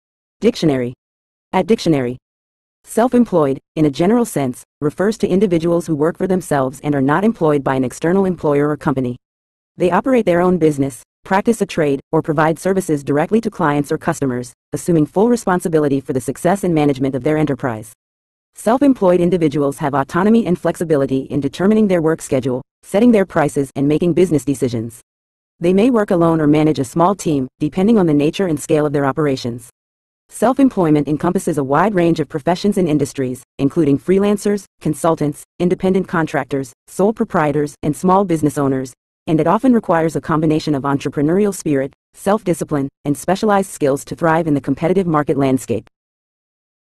Self-employed Meaning In English
Self-employed, in a general sense, refers to individuals who work for themselves and are not employed by an external employer or company. They operate their own business, practice a trade, or provide services directly to clients or customers, assuming full responsibility for the success and management of their enterprise. Self-employed individuals have autonomy and flexibility in determining their work schedule, setting their prices, and making business decisions. They may work alone or manage a small team, depending on the nature and scale of their operations. Self-employment encompasses a wide range of professions and industries, including freelancers, consultants, independent contractors, sole proprietors, and small business owners, and it often requires a combination of entrepreneurial spirit, self-discipline, and specialized skills to thrive in the competitive market landscape.